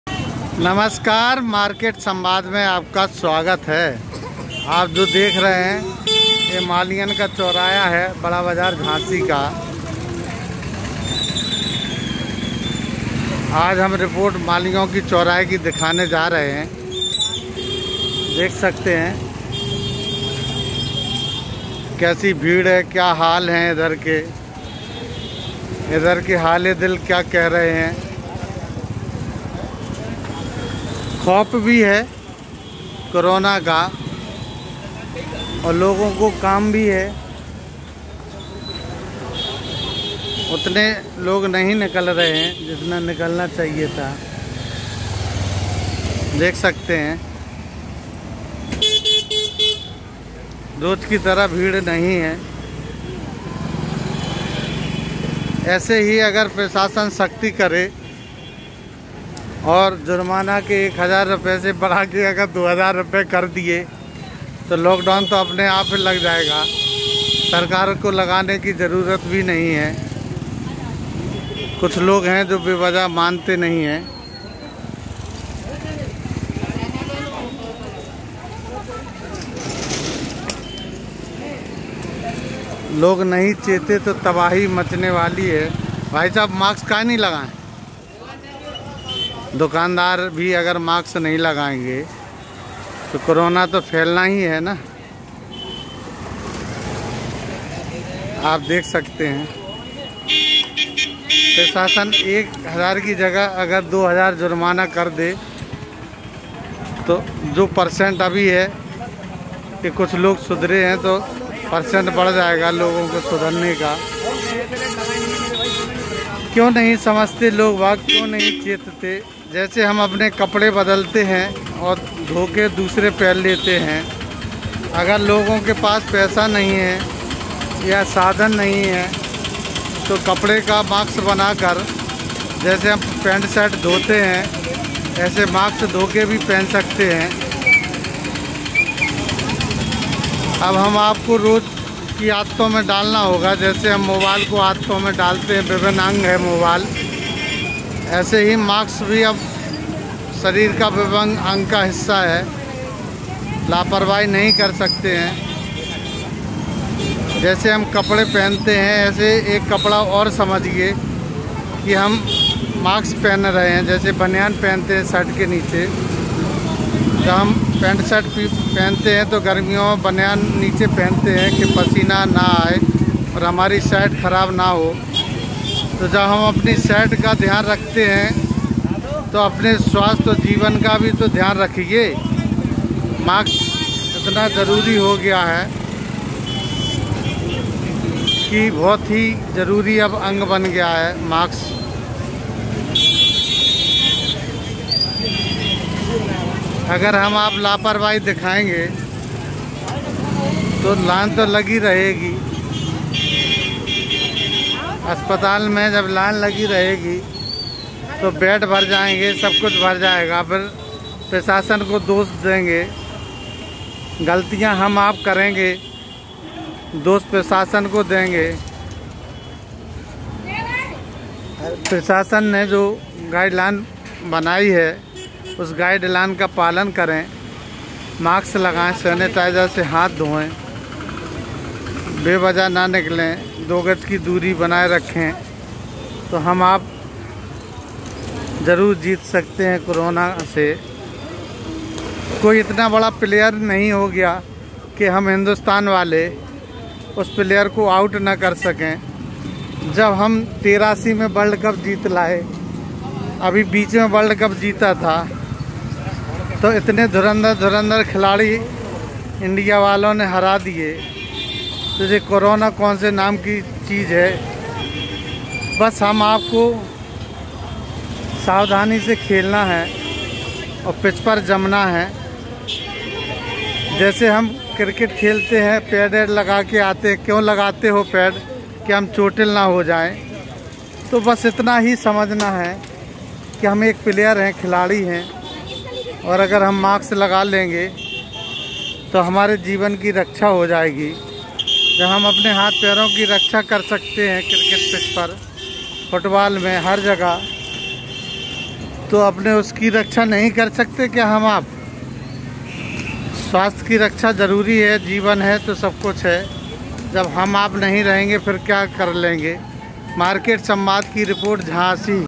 झाँसी - दिन में बड़ा बाज़ार की हलचल, रिपोर्ट-अनिल मौर्य
झांसी में कोरोना का कहर  जारी है यहां लाइट कर्फ़्यू  के साथ रविवार को  लॉकडाउन भी लगाया गया है । आज शनिवार की सुबह शहर का मुख्य बाजार, जिसे बड़ा बाजार  के नाम से जाना जाता है यहां पर लगभग लगभग सन्नाटे की स्थिति है । लोग कोरोना  की दहशत से बाजार में कम ही निकल रहे हैं